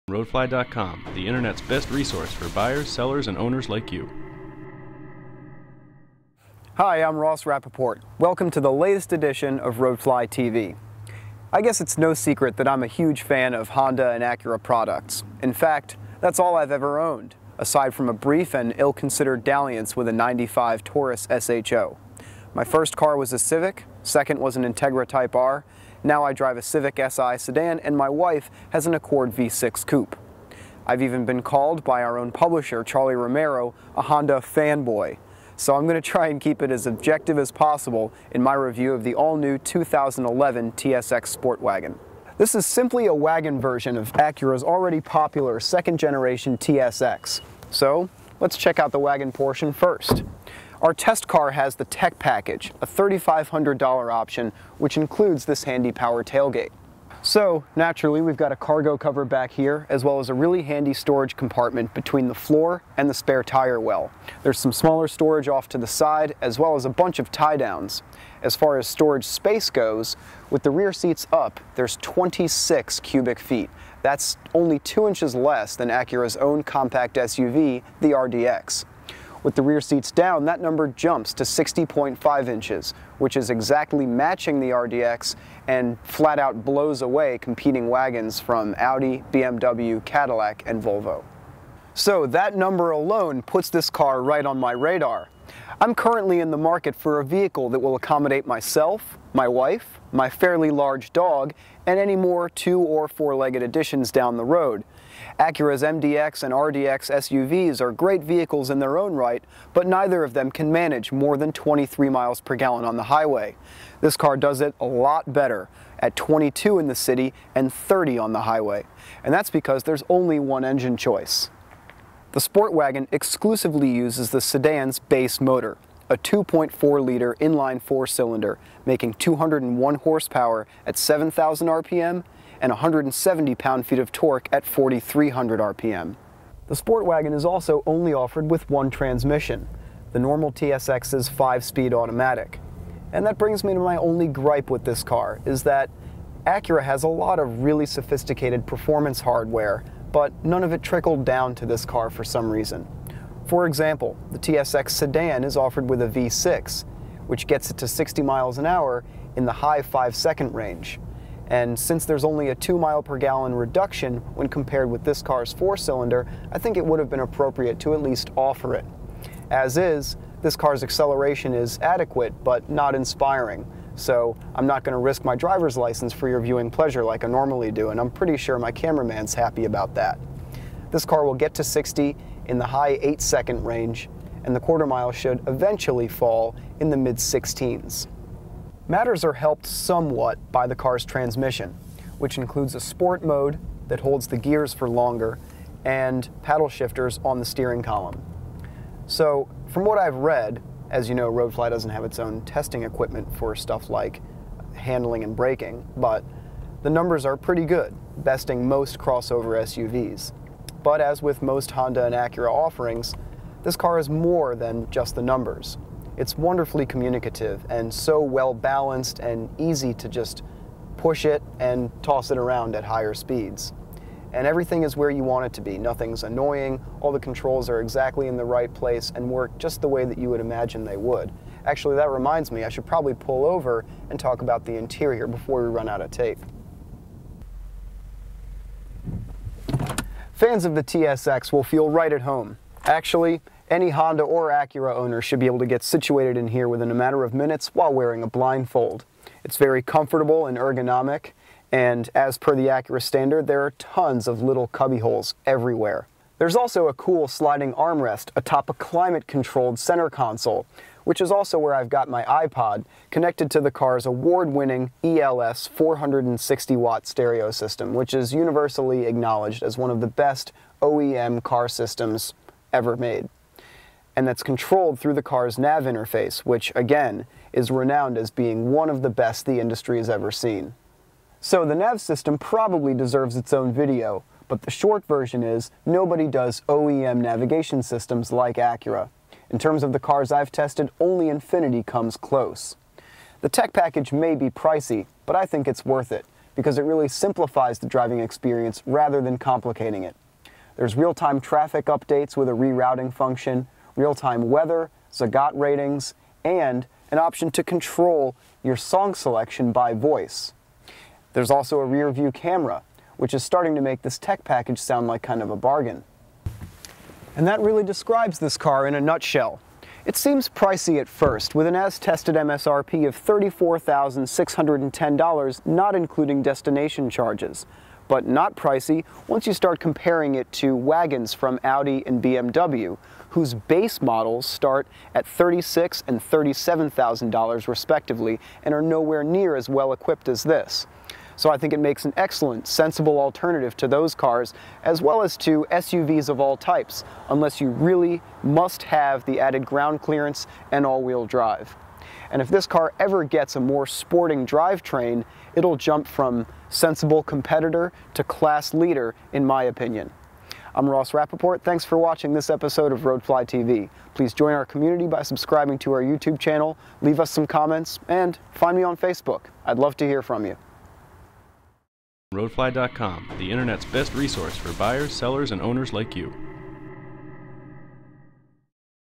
Roadfly.com - 2011 Acura TSX Sport Wagon w/Tech Package Road Test & Review
New for 2011, the TSX Sport Wagon adds an extra-strength dose of utility to Acura's entry-level model.

The addition of the wagon bodystyle gives the TSX more cargo room than competitors from Audi, BMW, and Volvo, at almost 26 cubic feet with the rear seats up, and over 60 cubic inches with the seats folded.

Powertrain options are limited: the TSX Sport Wagon comes only with the base 2.4L inline-4, paired with a 5-speed automatic.

That gets the car to 60mph in the mid-eight-second range, and delivers an EPA-rated 22 mpg city/30 mpg highway.

Priced at $34,610 (plus $860 destination charge), our fully loaded TSX Sport Wagon with Technology Package substantially undercut the starting MSRPs of competing wagons from Audi and BMW.  Its huge price advantage, combined with Acura's typically excellent reliability and resale value makes this wagon an extremely smart buy.

—Ross Rapoport, Road Test Editor & Video Host

To see additional pictures and the window sticker for the 2011 TSX Sport Wagon Tech Package,

Vehicle: 2011 Acura TSX Sport Wagon w/Technology Package
Base Price: $34,610
Price as tested: $35,470
Engine: DOHC 16v 2.4 inline four-cylinder
Transmission: 5-speed automatic w/Sequential SportShift
Horsepower: 201 hp @ 7,000 rpm
Torque: 170 lb-ft @ 4,300 rpm
0-60 MPH: 8.5 seconds
1/4-mile time: 16.5 sec. @ 84.9 mph
Warranty: 6 yr/70,000 mi powertrain warranty; 5 yr/60,000 mi limited vehicle warranty

Fuel economy (EPA miles per gallon rating): 22 city / 30 highway MPG